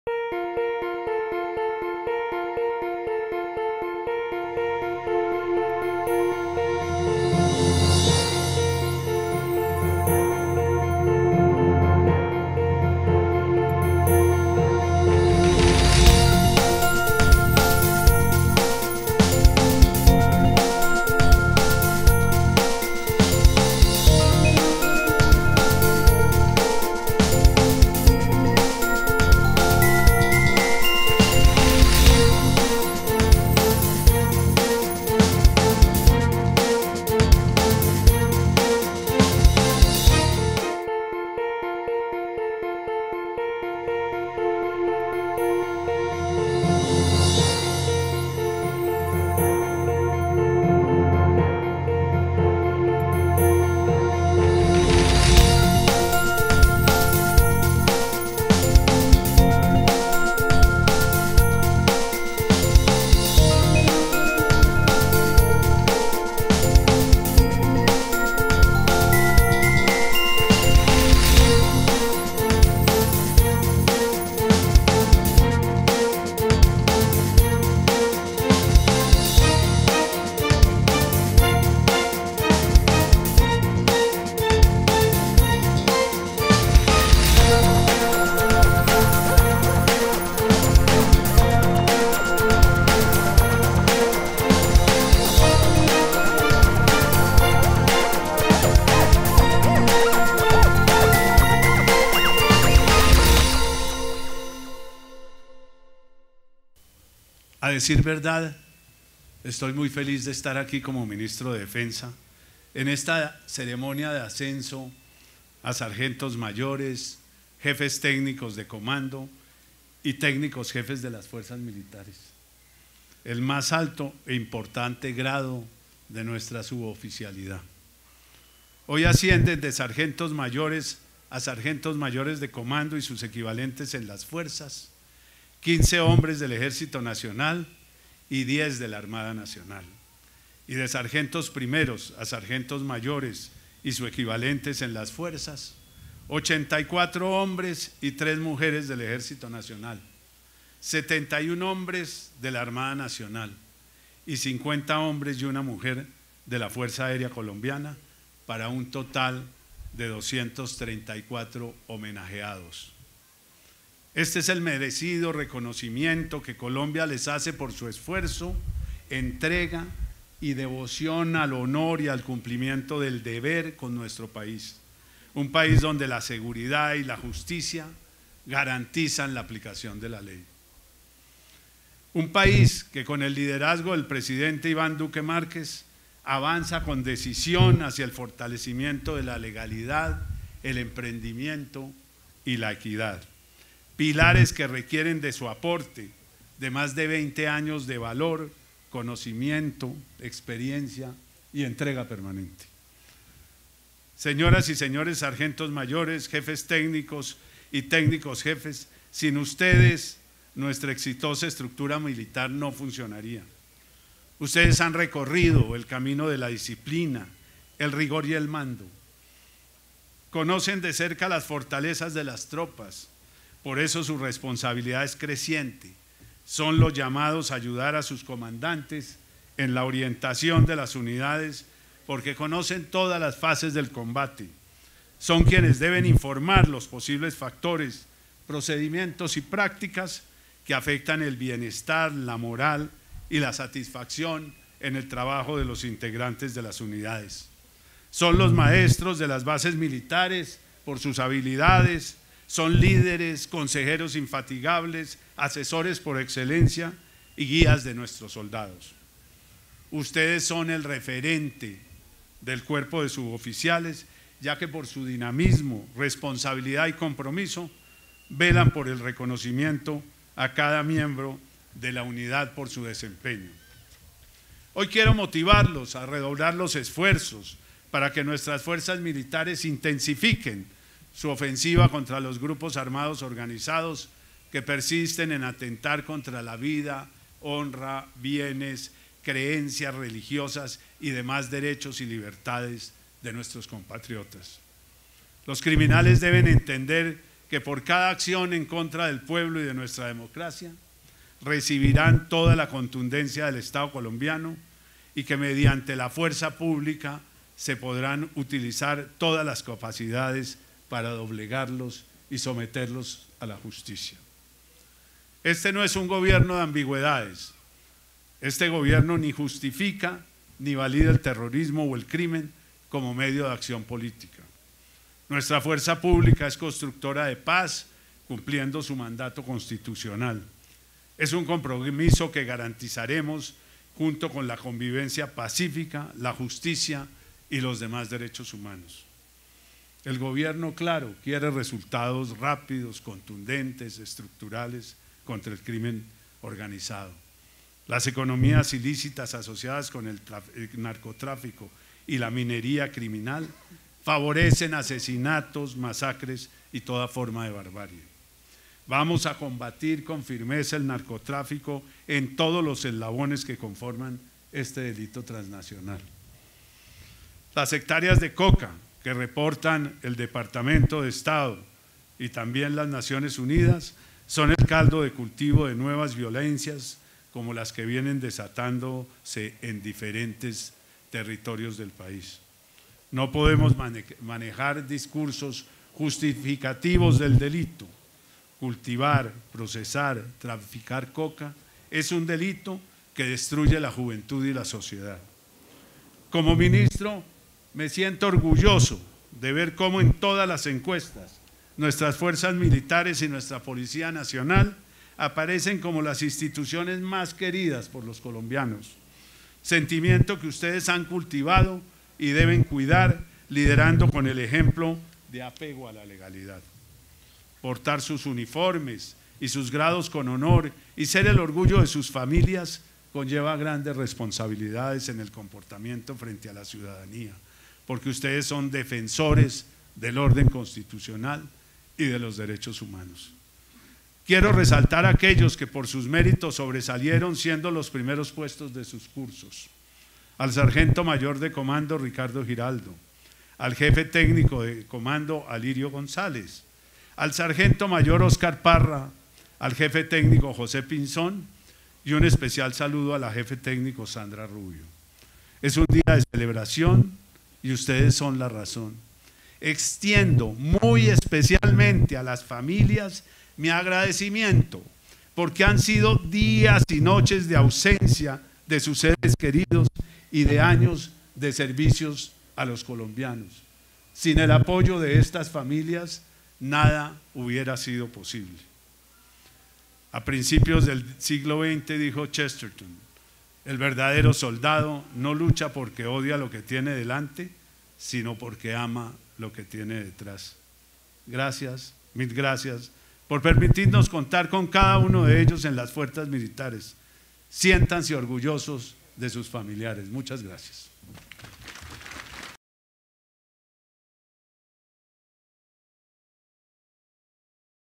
Vamos a combatir con firmeza el narcotráfico en todos los eslabones
En compañía de la cúpula militar, el ministro de Defensa, Guillermo Botero, presidió en la mañana de este martes, la ceremonia de ascenso de 234 suboficiales de las Fuerzas Militares, que alcanzaron su máximo grado como Sargentos Mayores, Jefes Técnicos y Técnicos Jefes.
Durante la ceremonia, el Ministro dijo que “las hectáreas de coca que reportan el Departamento de Estado de los EE. UU. y las Naciones Unidas son el caldo de cultivo de nuevas violencias como las que vienen desatándose en diferentes territorios del país. No podemos manejar discursos justificativos del delito, cultivar, procesar, traficar coca, es un delito que destruye la juventud y la sociedad”. Aseguró de igual manera que “el Gobierno quiere resultados rápidos, contundentes y estructurales contra el crimen organizado”.

El jefe de la cartera de Defensa motivó a los integrantes de las FF. MM. a “intensificar la ofensiva contra los Grupos Armados Organizados que persisten en atentar contra la vida, honra, bienes, creencias religiosas y demás derechos y libertades de nuestros compatriotas”.

El ministro Botero enfatizó que “los criminales deben entender que por cada acción en contra del pueblo y de la democracia, recibirán toda la contundencia del Estado colombiano y que mediante la Fuerza Pública se podrán utilizar todas las capacidades para doblegarlos y someterlos a la justicia. Este no es un Gobierno de ambigüedades, este Gobierno ni justifica, ni valida el terrorismo o el  crimen como medio de acción política”.

Al referirse al ascenso de los ascendidos, que alcanzaron su máximo grado en la suboficialidad, Botero les dijo que “su responsabilidad es creciente, son los llamados a ayudar a los comandantes en la orientación de las unidades, porque conocen todas las fases del combate (…).son los maestros de las bases militares por sus habilidades, son los líderes innatos, los consejeros incansables, asesores por excelencia y guías de nuestros soldados”.

En la ceremonia, llevada a cabo en el Círculo de Suboficiales de las Fuerzas Militares ascendieron de Sargentos Mayores a Sargentos Mayores de Comando y sus equivalentes en las Fuerzas, 15 hombres del  Ejército Nacional y 10 de la Armada Nacional; y de Sargentos Primeros a Sargentos Mayores y sus equivalentes en las Fuerzas, 84 hombres y 3 mujeres del Ejército Nacional; 71 hombres de la Armada Nacional, y 50 hombres y una mujer de la Fuerza Aérea Colombiana, para un total de 234.